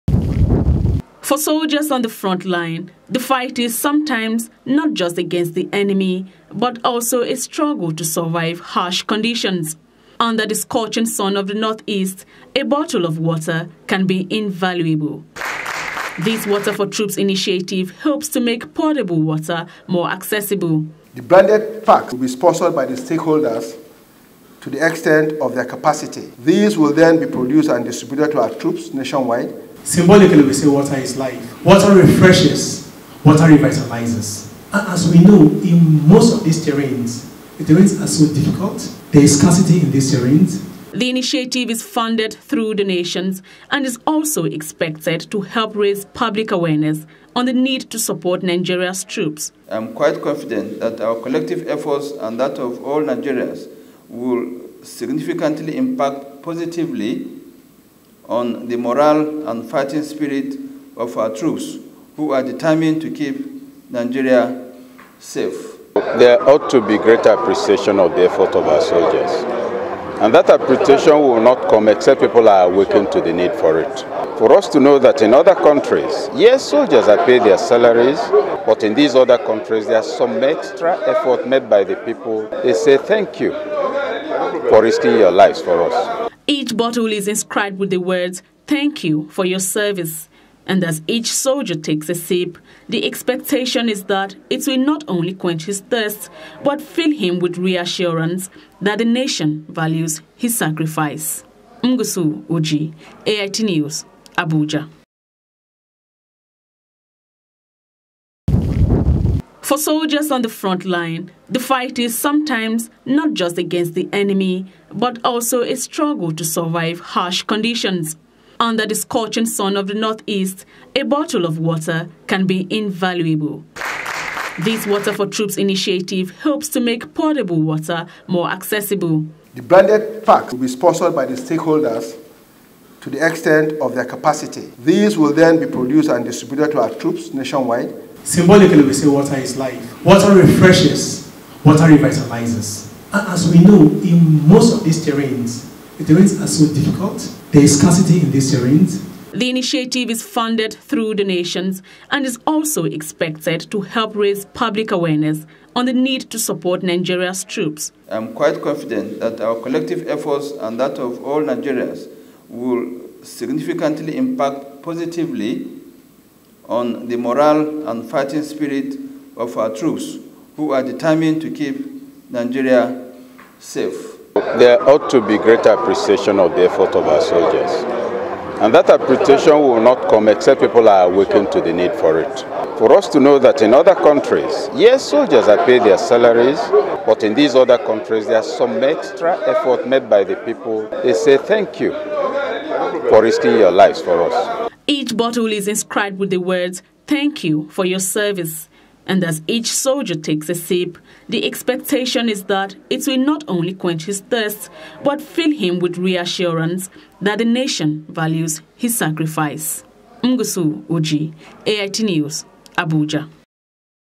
WATER FOR TROOPS
For the first time ever, Nigerians are coming together to show their appreciation to troops fighting terrorism in a tangible way. Bottles of water, branded with "Thank you for your service" are being sent to troops in the frontlines, to support government provision as well as encourage those risking their lives to protect the country against terror.